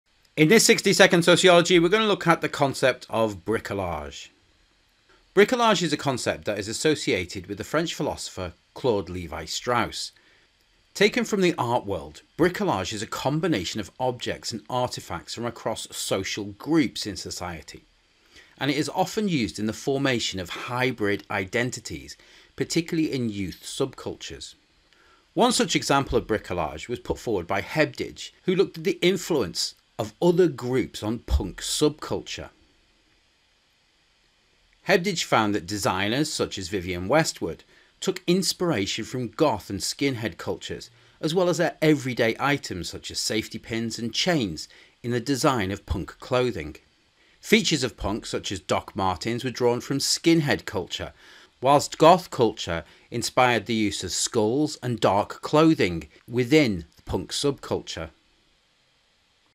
Bricolage | 60 Second Sociology (Culture and Identity)
This quick summary video outlines what is meant by the term bricolage, as used in the A Level Sociology topic of Culture & Identity.